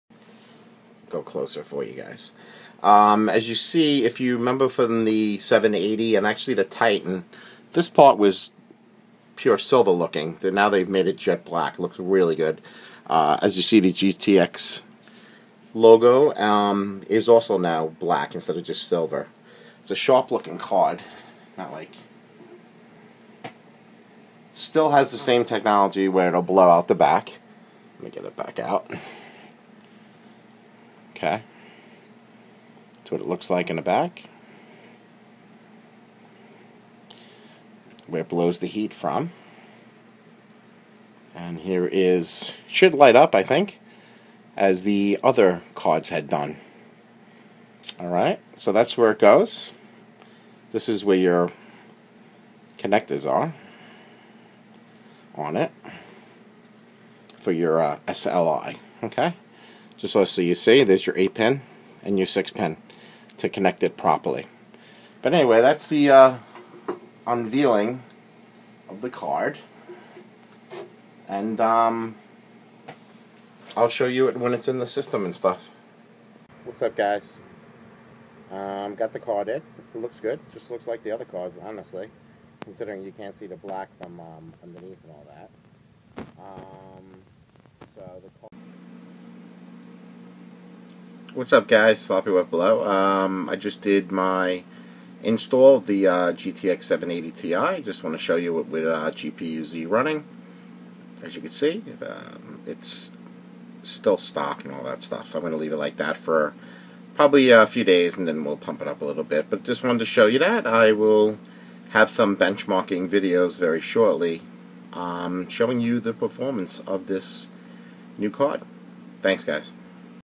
EVGA GEFORCE GTX 780 Ti ** UNBOXING & HANDS-ON**PART 2
NVIDIA GeForce GTX 780 Ti Specifications:
Graphics Processing Clusters
5
Streaming Multiprocessors
15
CUDA Cores (single precision)
2880
CUDA Cores (double precision)
960
Texture Units
240
ROP Units
48
Base Clock
875MHz
Boost Clock
928MHz
Memory Clock (Data rate)
7000 MHz
L2 Cache Size
1536K
Total Video Memory
3072MB GDDR5
Memory Interface
384-bit
Total Memory Bandwidth
336 GB/s
Texture Filtering Rate (Bilinear)
210 GigaTexels/sec
Fabrication Process
28 nm
Transistor Count
7.1 Billion
Connectors
2 x Dual-Link DVI 1 x HDMI 1 x DisplayPort
Form Factor
Dual Slot
Power Connectors
One 8-pin and one 6-pin
Recommended Power Supply
600 Watts
Thermal Design Power (TDP)
250 Watts
Thermal Threshold
95° C